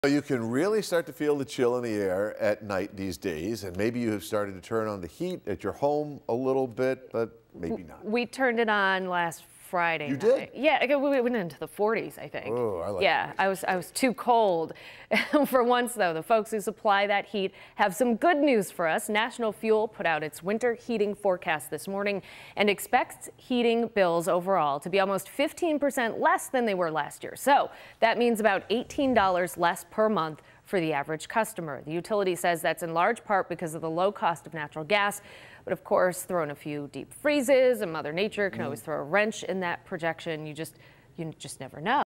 National Fuel Expects Lower Heating Bills
expects heating bills overall to be almost 15-percent less than they were last year.